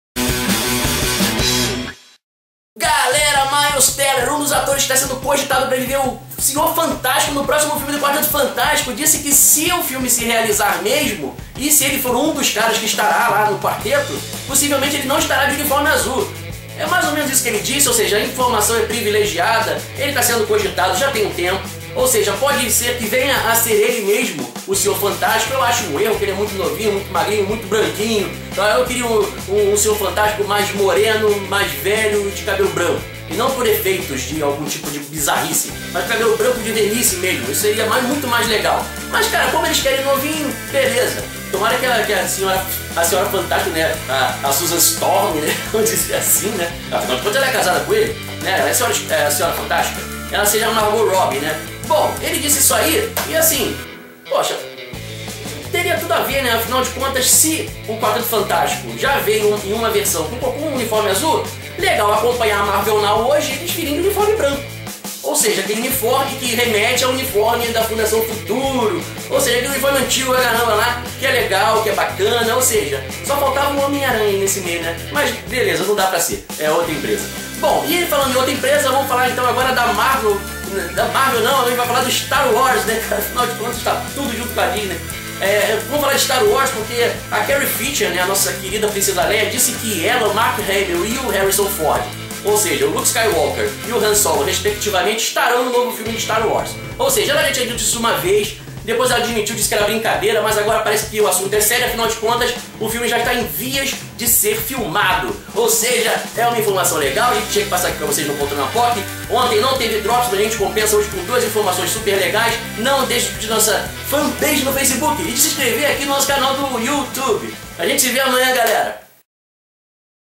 DROPS #79 - Quarteto de Branco e Leia Organa em Star Wars VII | Poltrona Pop
Comentamos o rumor espalhado por Miles Teller, um dos atores no páreo para viver
Reed Richards, o Sr. Fantástico do reboot de Quarteto Fantástico. Não poderíamos deixar de falar também do pronunciamento de Carrie Fisher sobre ela, Mark Hamill e Harrison Ford estarem confirmados em Star Wars VII, de J.J. Abrams.